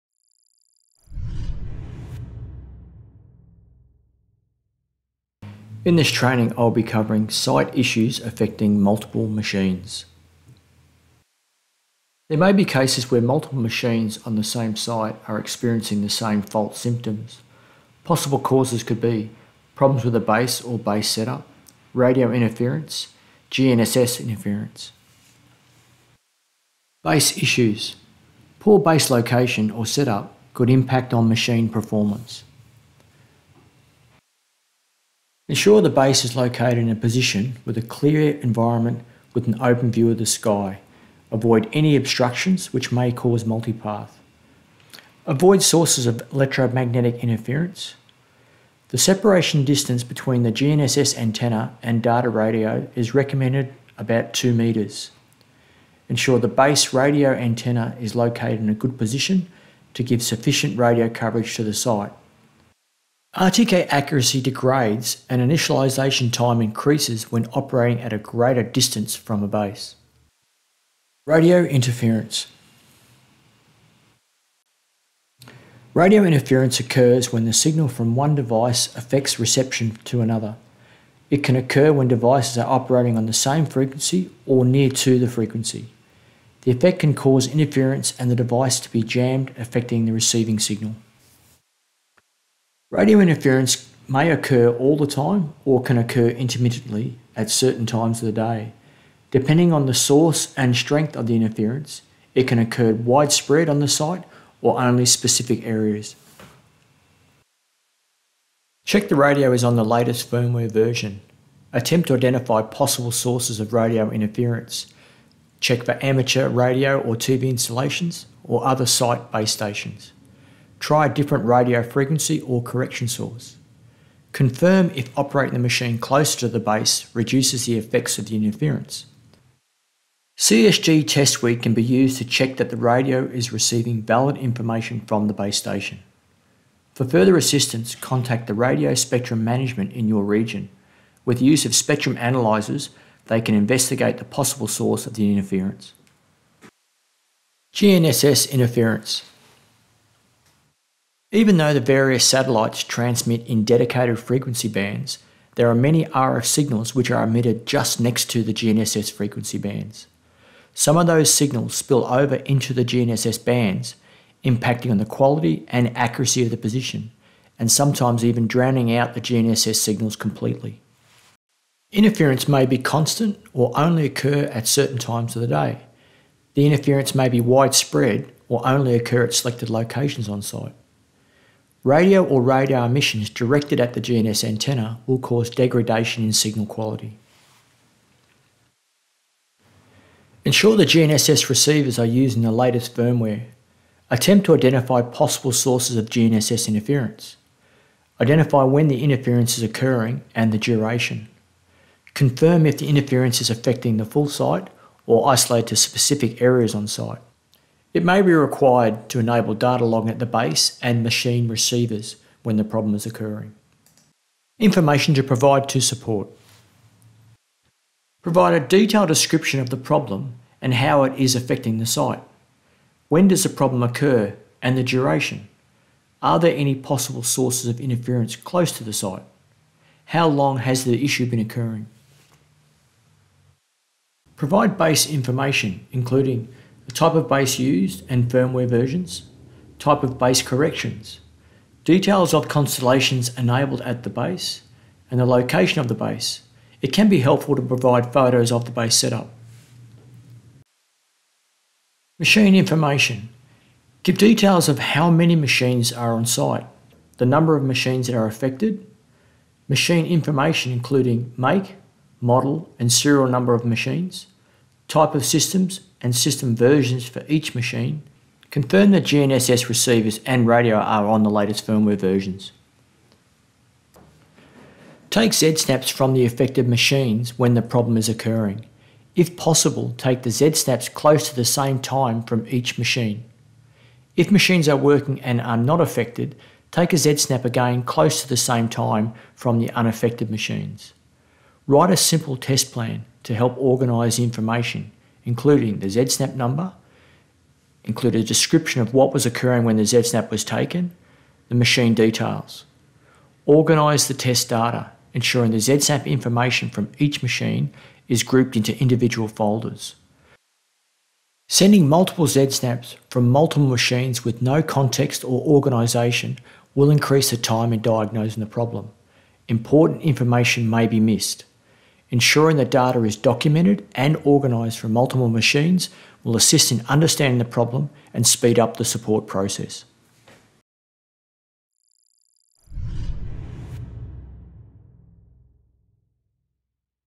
Trimble Earthworks: Troubleshooting - Site issues affecting multiple machines
This video will cover collecting data to diagnose problems due to base, problems caused by interference/multipath etc.